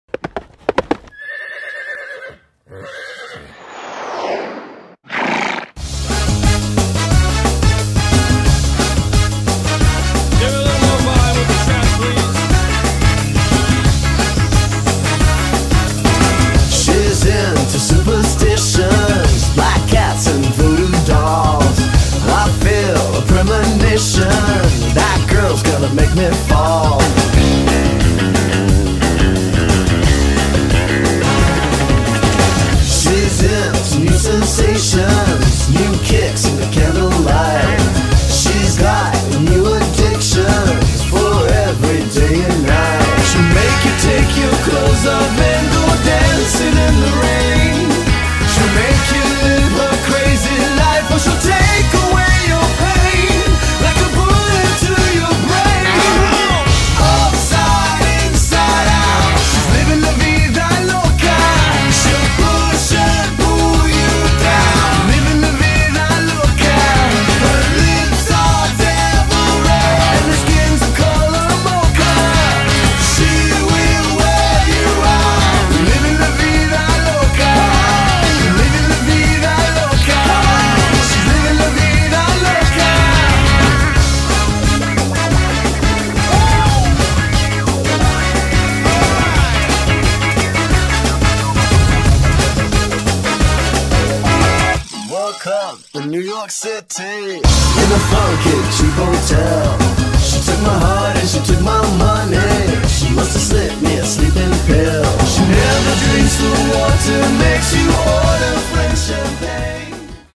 115S Abigail Read at Ram Tap Horse Trials Stadium Jumping Eventing May 2010
115S Abigail Read at Ram Tap Horse Trials Stadium Jumping Eventing May 2010